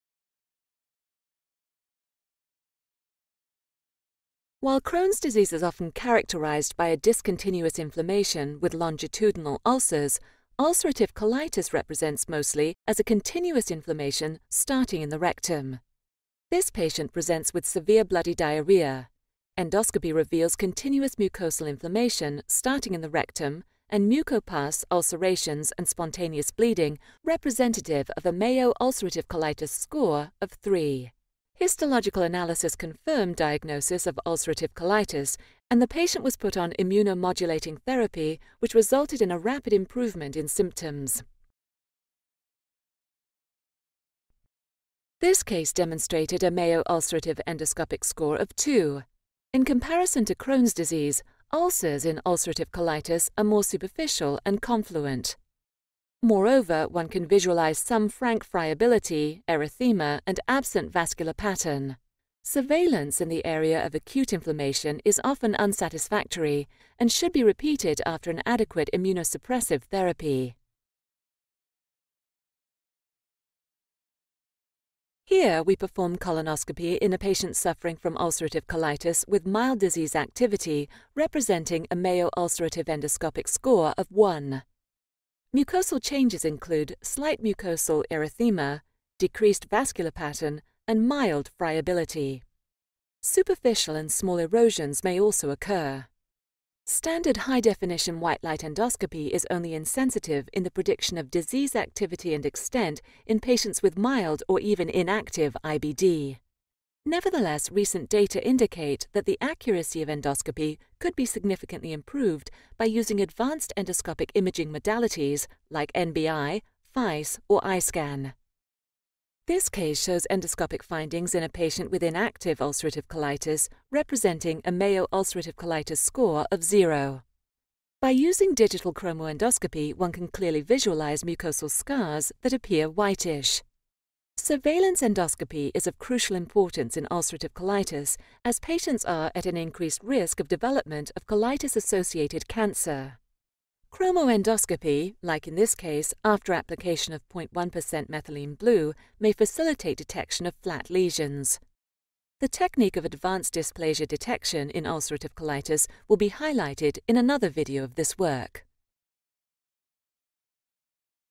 Endoscopic Findings in Ulcerative Colitis
Abstract: Patients with long-standing ulcerative colitis are at an increased risk for the development of intraepithelial neoplasia and colitis-associated cancer, and guidelines recommend colonoscopy as an outcome parameter for biological therapies or to assess therapeutic efficacy.

This video highlights the various endoscopic findings in different disease stages of ulcerative colitis. The technique of advanced dysplasia detection in ulcerative colitis will be highlighted in another video of this library. This article is part of an expert video encyclopedia.